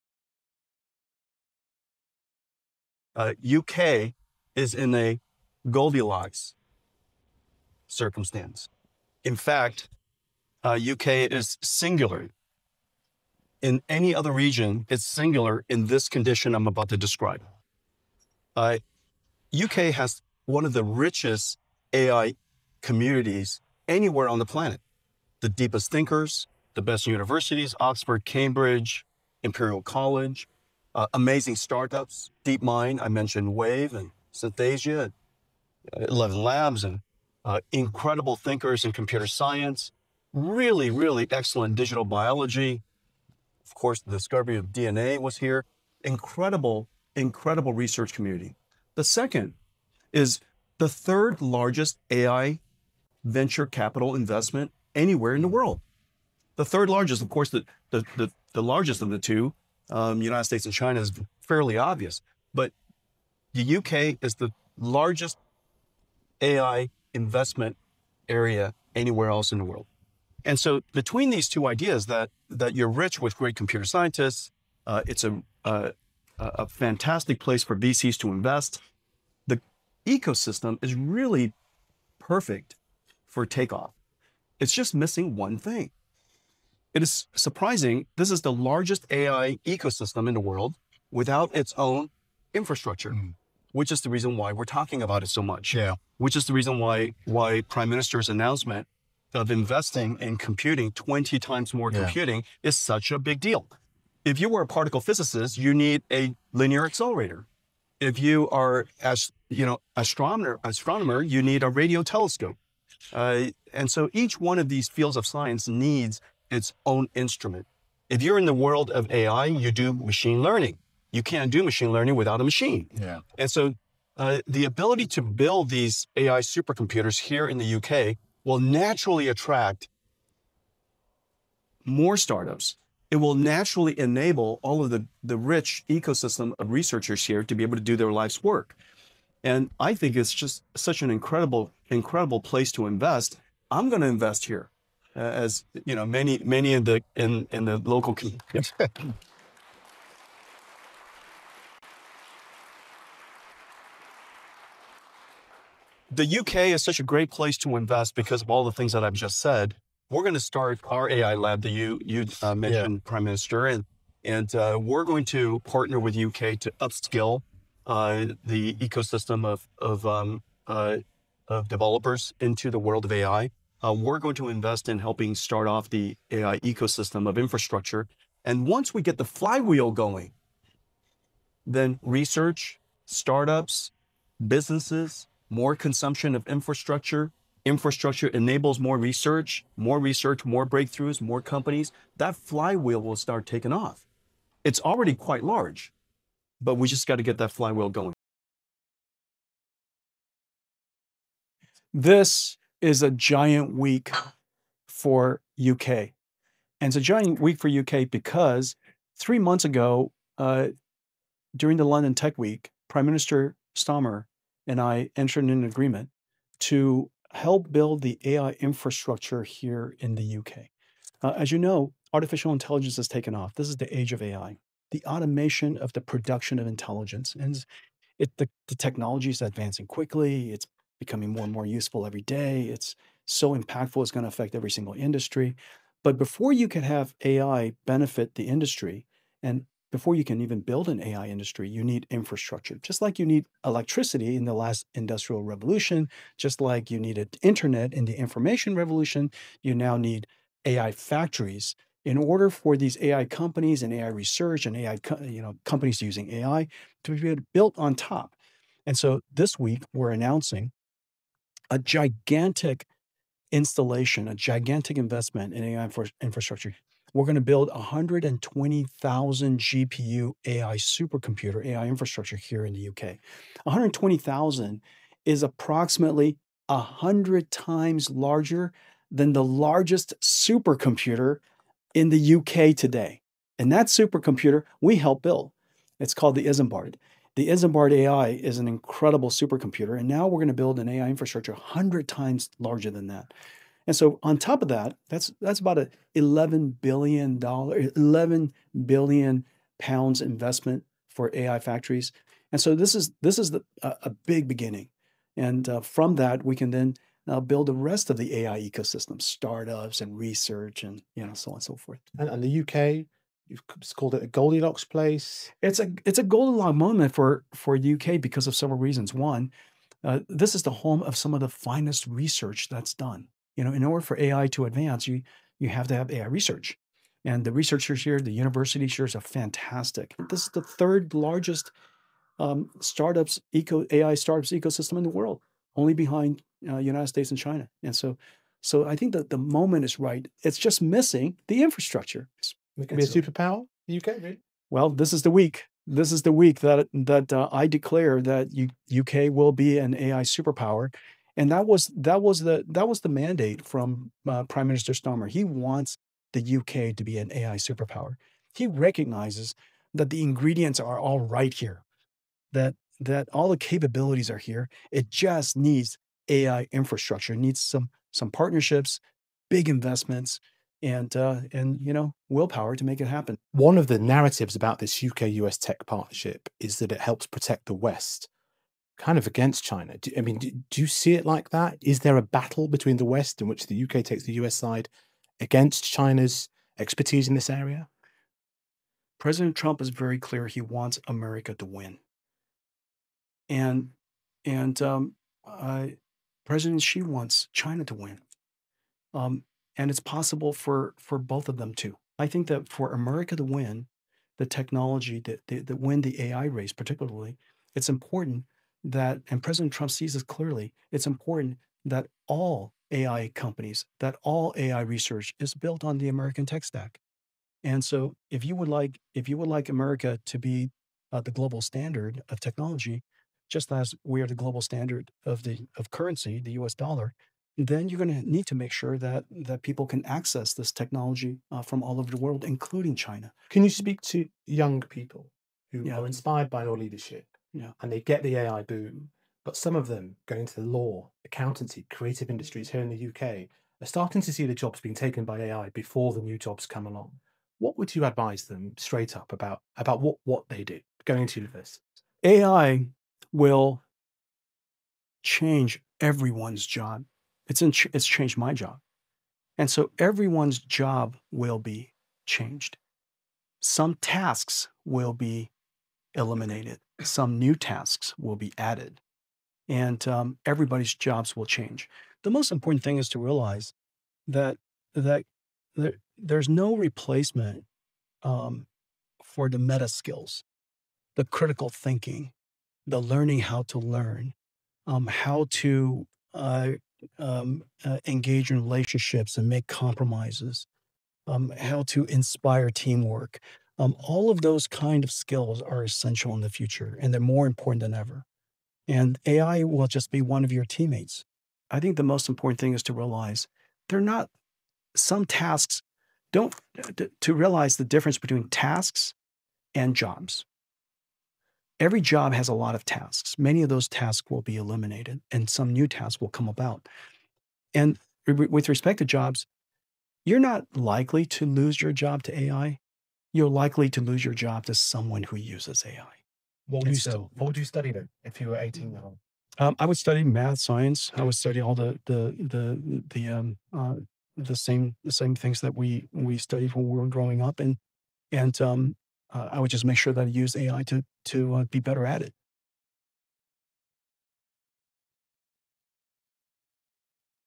黃仁勳：英國的AI布局與願景｜Jensen Huang: UK in 'Goldilocks' Position for AI
NVIDIA founder Jensen Huang has just delivered major news in the UK: from praising Britain’s vibrant AI ecosystem to announcing a £11 billion investment to build a 120,000-GPU “AI Factory.” How does he view UK-US cooperation, global tech competition, and AI’s impact on the next generation? This video breaks down the “UK AI Golden Moment” and what it means for the future.

NVIDIA創辦人黃仁勳最近在倫敦科技週上表示，英國正處於“金發姑娘環境”，因為英國擁有“全球最富有的人工智能社區之一”，擁有“最深刻的思想家和最優秀的大學”，但需要更多基礎設施來釋放其潛力。 「這是世界上最大的、沒有自身基礎設施的人工智慧生態系統」。

黄仁勋隨特朗普訪問英國時連發重磅訊息：從盛讚英國AI生態系統到宣布投資高達110億英鎊（折合近159億美元）建設12萬GPU超級計算「AI工廠」，他如何看待英美合作、地緣競爭、以及AI對年輕人的影響？本片帶你一次看懂「英國AI黃金時刻」背後的布局與願景。